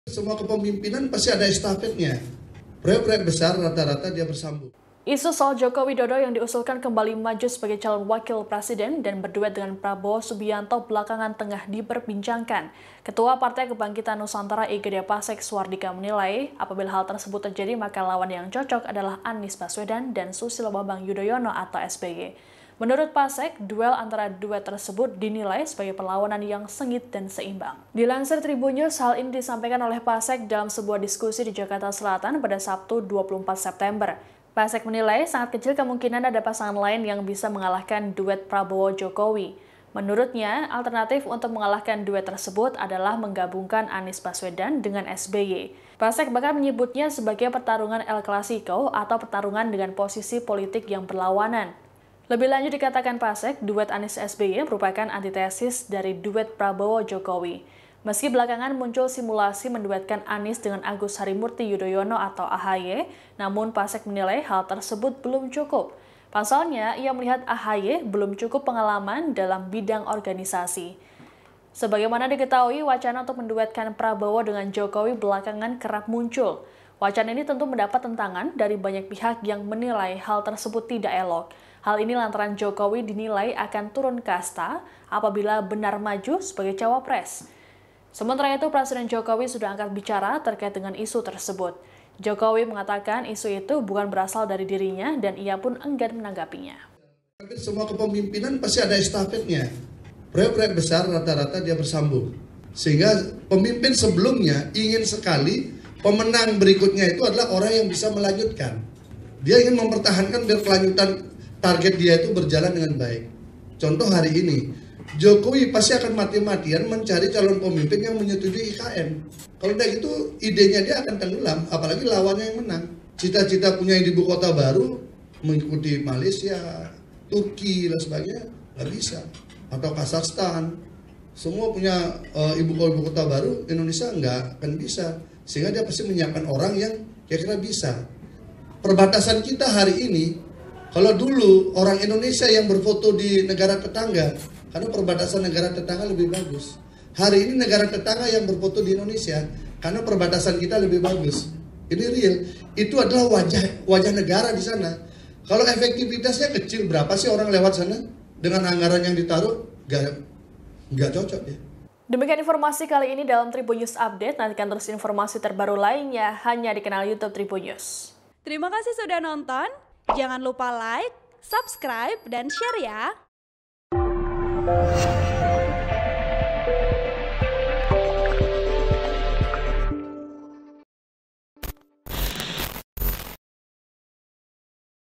Wacana Duet Anies-SBY Dinilai Pas untuk Lawan Prabowo-Jokowi, Pasek: Pertarungan El Clasico
TRIBUN-VIDEO.COM - Isu soal Joko Widodo (Jokowi) yang diusulkan kembali maju sebagai calon wakil presiden dan berduet dengan Prabowo Subianto belakangan tengah diperbincangkan.

Ketua Partai Kebangkitan Nusantara, I Gede Pasek Suardika menilai, apabila hal tersebut terjadi, maka lawan yang cocok adalah Anies Baswedan dan Susilo Bambang Yudhoyono (SBY).

Menurut Pasek, duel antara duet tersebut dinilai sebagai perlawanan yang sengit dan seimbang.

Dilansir Tribunnews, hal ini disampaikan oleh Pasek dalam sebuah diskusi di Jakarta Selatan pada Sabtu (24/9).

Pasek menilai, sangat kecil kemungkinan ada pasangan lain yang bisa mengalahkan duet Prabowo-Jokowi.

Menurutnya, alternatif untuk mengalahkan duet tersebut adalah menggabungkan Anies Baswedan dengan SBY.

Pasek bahkan menyebutnya sebagai pertarungan El Clasico atau pertarungan dengan posisi politik yang berlawanan.

Kita kan lagi dengar nih isu Prabowo-Jokowi, nah ini momentum bagus juga Pak SBY bisa jadi wakil presidennya siapa," ucap Pasek dalam sebuah diskusi di Jakarta Selatan, Sabtu (24/9/2022).

"Nah, kalau Pak Prabowo-Jokowi maju, siapa yang bisa jadi lawan seru? Maka yang memungkinkan ya Anies-SBY, ini baru akan ada pertarungan yang... El Classico lah," ujar mantan politikus Partai Demokrat itu.

Lebih lanjut dikatakan Pasek, duet Anies-SBY merupakan antitesis dari duet Prabowo-Jokowi.

Meski belakangan muncul simulasi menduetkan Anies dengan Agus Harimurti Yudhoyono (AHY), namun Pasek menilai hal tersebut belum cukup.

Pasalnya, ia melihat AHY belum cukup pengalaman dalam bidang organisasi.

Sebagaimana diketahui, wacana untuk menduetkan Prabowo dengan Jokowi belakangan kerap muncul.

Wacana ini tentu mendapat tentangan dari banyak pihak yang menilai hal tersebut tidak elok.

Hal ini lantaran Jokowi dinilai akan turun kasta apabila benar maju sebagai cawapres.

Sementara itu, Presiden Jokowi sudah angkat bicara terkait isu tersebut.

Jokowi mengatakan isu tersebut bukan berasal dari dirinya, dan ia pun enggan menanggapinya.

(TribunVideo.com)

Artikel ini telah tayang di Tribunnews.com dengan judul Gede Pasek Sebut Anies-SBY Cocok Lawan Prabowo-Jokowi untuk Pilpres 2024: El Classico,
Penulis: Ilham Rian Pratama
Editor: Wahyu Aji